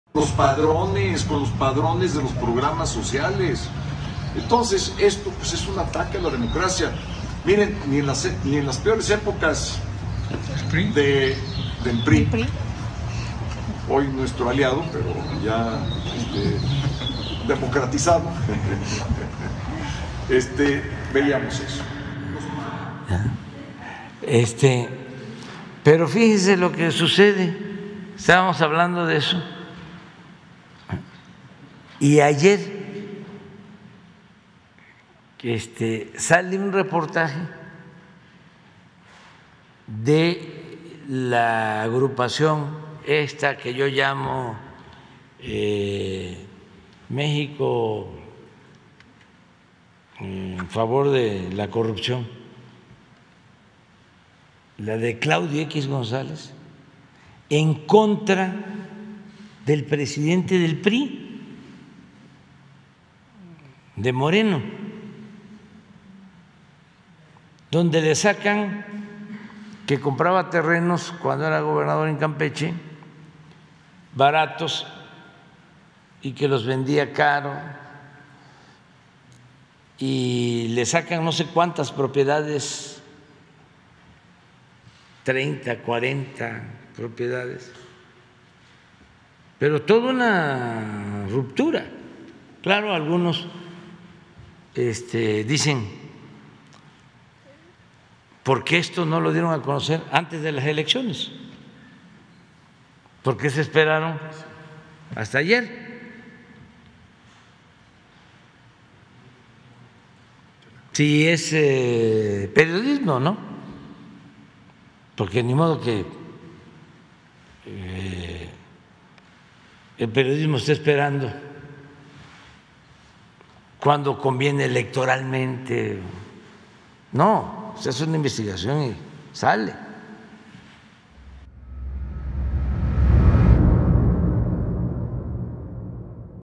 AMLO exhibe la doble moral de la alianza del Va Por México
Video El presidente López Obrador exhibió la doble moral de la oposición y destacó como claudioxgonzalez busca ya deshacer del PRI, “Ahí si hay tiro” AMLO👇👇👇